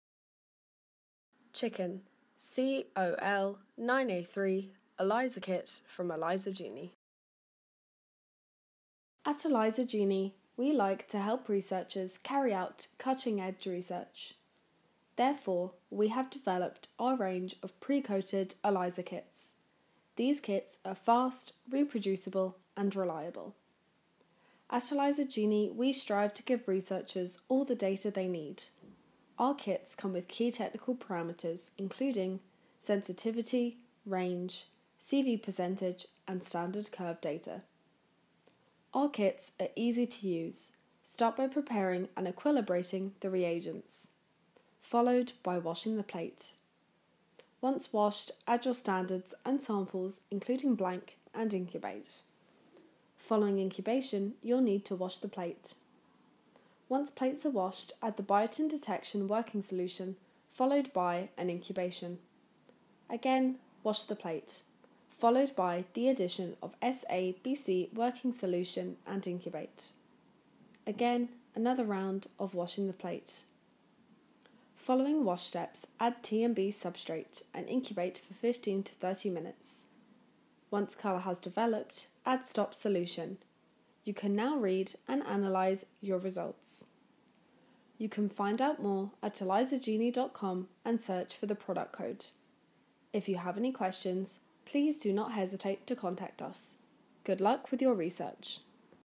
Chicken Collagen alpha-3(IX) chain (COL9A3) ELISA Kit (SKU: CHEB0312)
The ELISA Genie Chicken Collagen alpha-3(IX) chain (COL9A3) ELISA Kit can assay for Chicken Collagen alpha-3(IX) chain in the following samples: serum, blood, plasma, cell culture supernatant and other related supernatants and tissues.